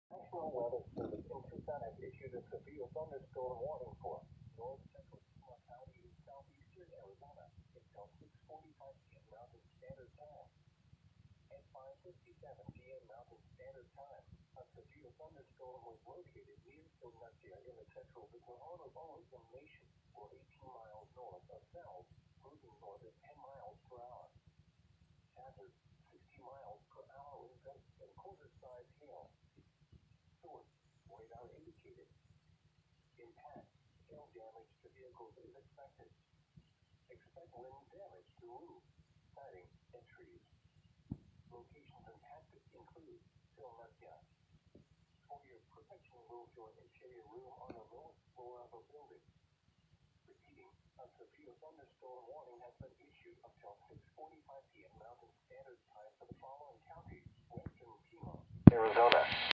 NOAA Weather Radio (Tucson,AZ)(Severe Thunderstorm Warning) 6/1/2020
Severe Thunderstorm Warning AZC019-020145- /O.NEW.KTWC.SV.W.0004.200602T0058Z-200602T0145Z/ BULLETIN - IMMEDIATE BROADCAST REQUESTED Severe Thunderstorm Warning National Weather Service Tucson AZ 558 PM MST Mon Jun 1 2020

The National Weather Service in Tucson has issued a * Severe Thunderstorm Warning for... North central Pima County in southeastern Arizona... * Until 645 PM MST.

* At 557 PM MST, a severe thunderstorm was located near Sil Nakya in the central Tohono Oodham Nation, or 18 miles north of Sells, moving north at 10 mph. HAZARD...60 mph wind gusts and quarter size hail. SOURCE...Radar indicated. IMPACT...Hail damage to vehicles is expected. Expect wind damage to roofs, siding, and trees.

* Locations impacted include... Sil Nakya.

PRECAUTIONARY/PREPAREDNESS ACTIONS... For your protection move to an interior room on the lowest floor of a building.

&& LAT...LON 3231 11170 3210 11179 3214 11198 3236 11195 TIME...MOT...LOC 0057Z 194DEG 8KT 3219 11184

 HAIL...1.00IN WIND...60MPH